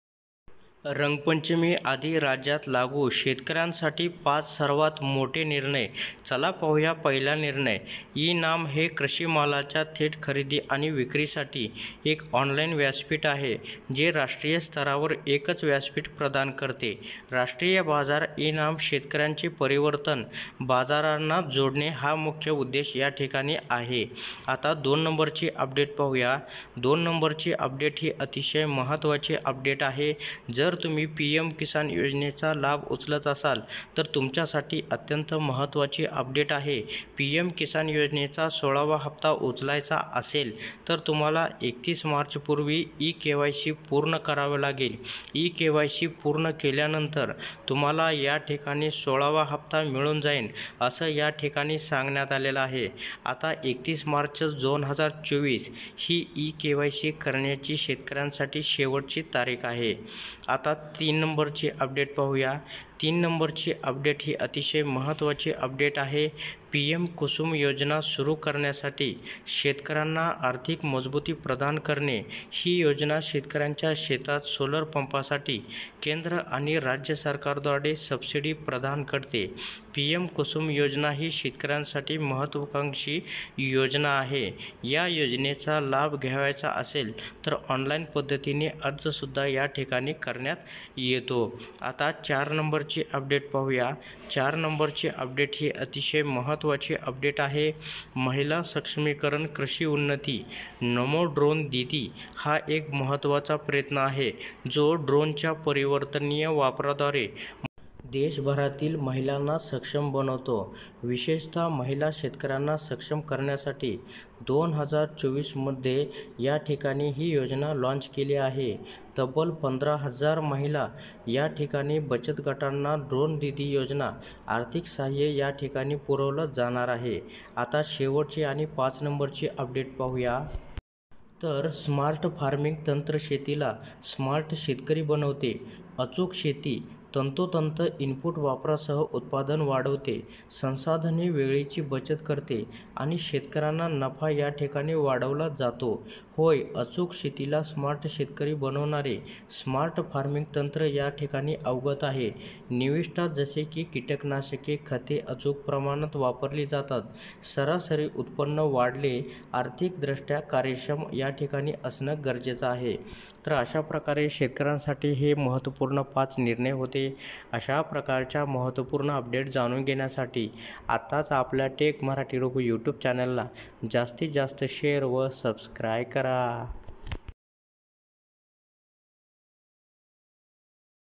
रंगपंचमी आधी राज्यात लागु | शेतकर्‍यांसाठी 5 मोठे निर्णय | 30 मार्च 2024 | Shetkari Batmi Today
हे पण Video पहा..

धन्यवाद.